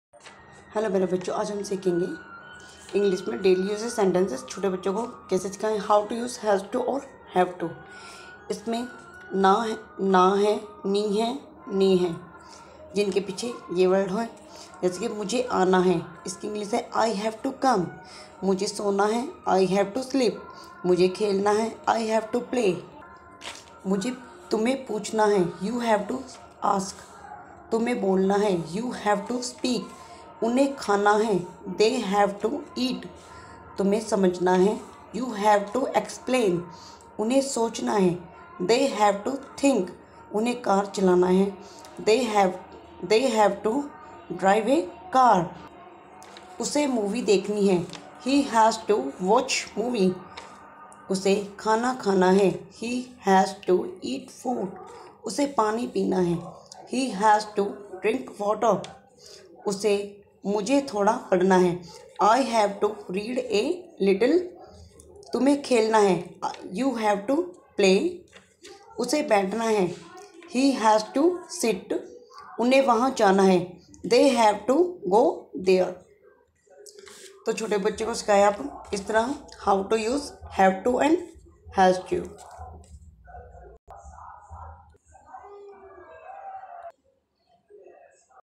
|| HOW TO USE HAS TO/ HAVE TO || CLASS - 2nd To 5th
hi
in this video I am showing about How to teach young children to in English How to use has to/ have to easily write with learn for kids.